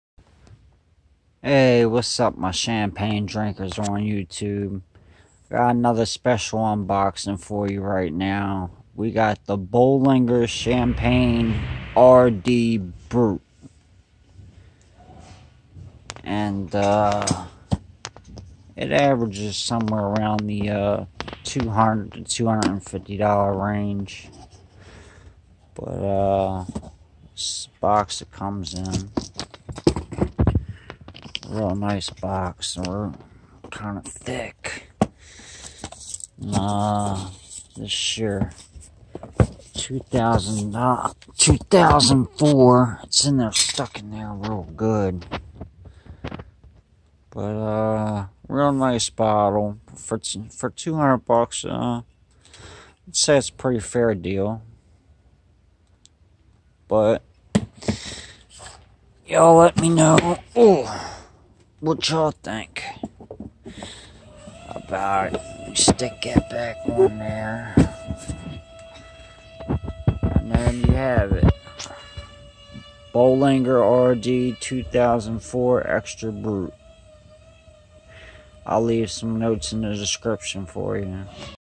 Bollinger R.D. extra brut 2004 unboxing review
The highly exclusive, world-class Bollinger R.Dis made from a blend of 66% Pinot Noir and 34% Chardonnay.

A 88% of the grapes used to produce this Vintage Champagne come from Grand Cru vineyards. The remaining 12% were selected among the best Premier Cru plots.

The base wine was fermented in old French oak barrels previously used to age Chardonnay wines.

Then this Champagne by the Maison Bollingerwas bottled and aged in the cellar for 96 months.

The letters R. D. mean "Récemment dégorgé" (recently disgorged). Thus, if we wish we can keep Bollinger R.D 2004 and age it in our cellar for a minimum of 10-12 years.

Appearance

This Champagne from Reims has a pale, clean yellow colour with some grey tinges.

Nose

Fabulously refined, this blend of Pinot Noir and Chardonnayis really in a class of its own. Bollinger R. D suggests aromas of sugar-coated white fruit, apple compote, quince jelly and honey, nicely underpinned by some extremely subtle, understated notes of toast, aniseed, nutmeg, cocoa shell and withered flowers.

Palate

Breathtakingly elegant and seductive, this Champagne by Maison Bollinger has real complexity, with focus and a great confidence. Caressing bubbles, exceptional acidity and a kaleidoscopic complexity which would be painful reducing to words. Endless, dreamy finish.

Our personal opinion

A monument of a wine,Bollinger R.D 2004 is amazingly deep and exhilarating.

OTHER VINTAGES

Bollinger R.D. 2002:

Enterwine 96. Parker 92.

Year2004Type of Sparkling WineExtra BrutDenomination of Origin (DO)ChampagneBottle size75 clServing tempurature9-11ºCAgeingGran MillésiméNumber of months aged in the bottle96Parker's Score97Enterwine's Score96Media rating97Percentage of varietyPinot Noir 66%, Chardonnay 34%.PairingsAlone, Baked whitefish, Boullabaise, Caviar, Curred fish, Fish and gravey, Foie Gras, Grilled fish, Oysters, Soup and rice, Stew, Whitefish saladsTasteComplex and Elegant, Creamy with fine bubbles, Very intense and inkyOccasionA special occasion, Because you deserve it, Romantic Dinner